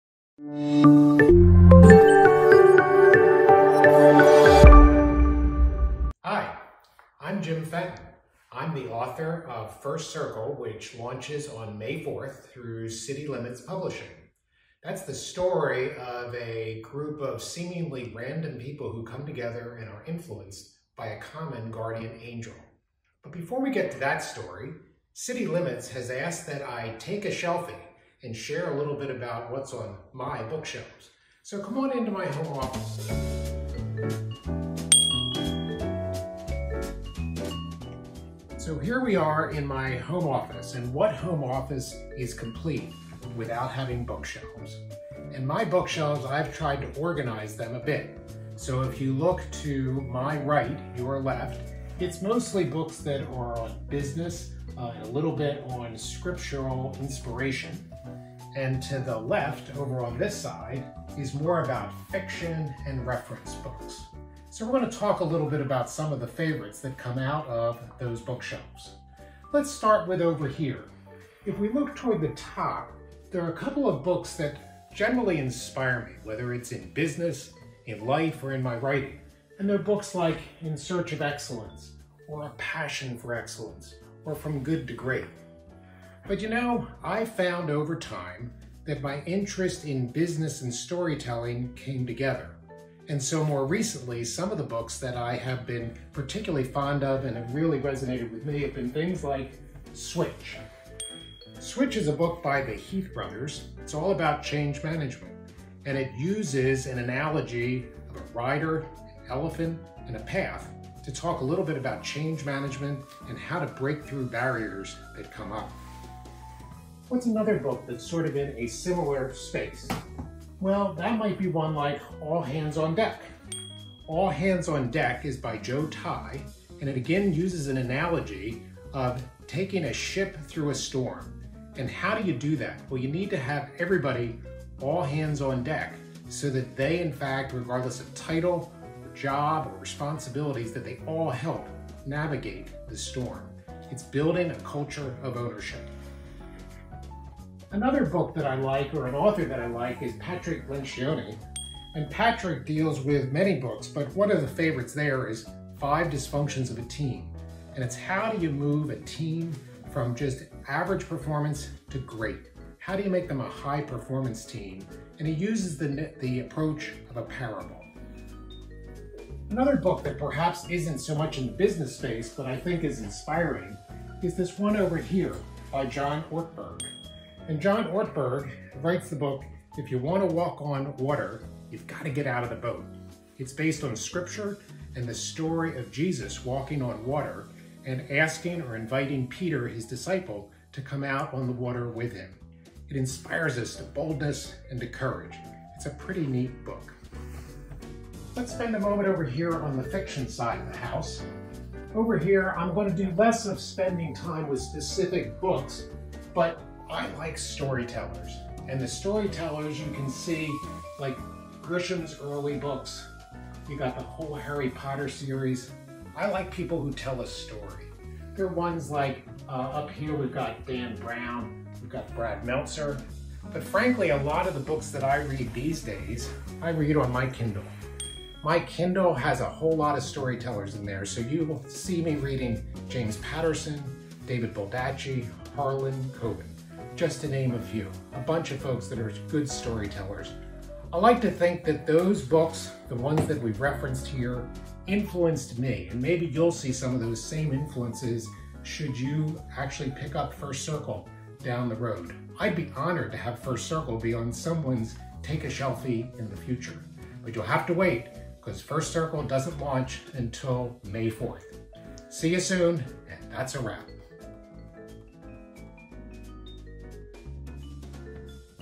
TakesShelfie with Jim Fenton
Join author Jim Fenton as he takes us on a tour of his stunning bookshelf at home and shows us the books that have inspired him the most, and works by his favorite story tellers. 📚